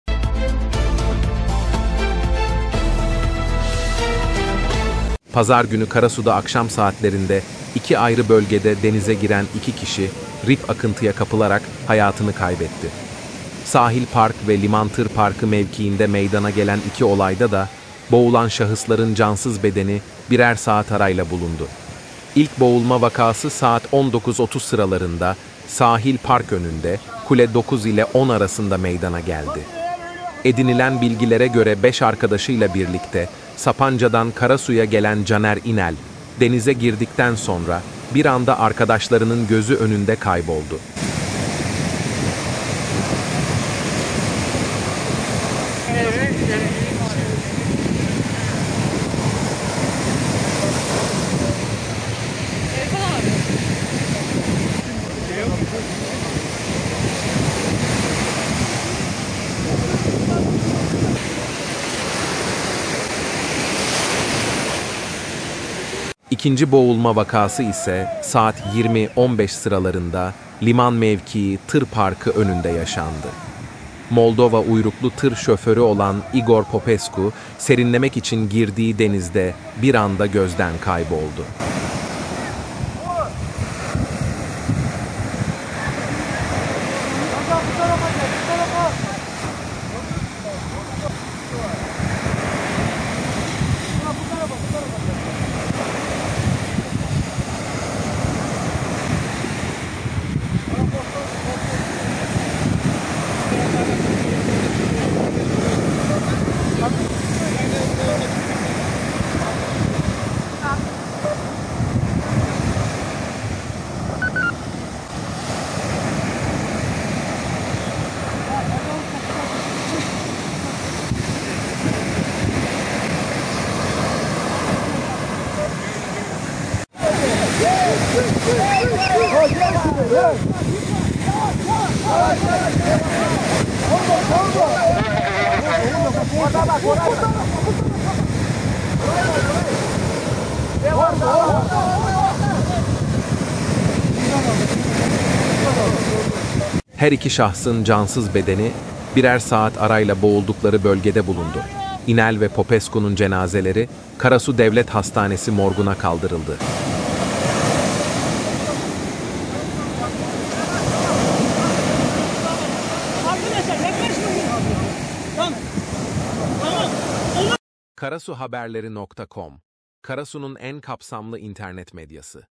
Karadeniz, Karasu'da bir saatte iki can aldı
Pazar günü Karasu’da akşam saatlerinde iki ayrı bölgede denize giren iki kişi, RİP akıntıya kapılarak hayatını kaybetti. Sahil Park ve Liman tır parkı mevkiinde meydana gelen iki olayda da, boğulan şahısların cansız bedeni birer saat arayla bulundu. - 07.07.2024 - HABER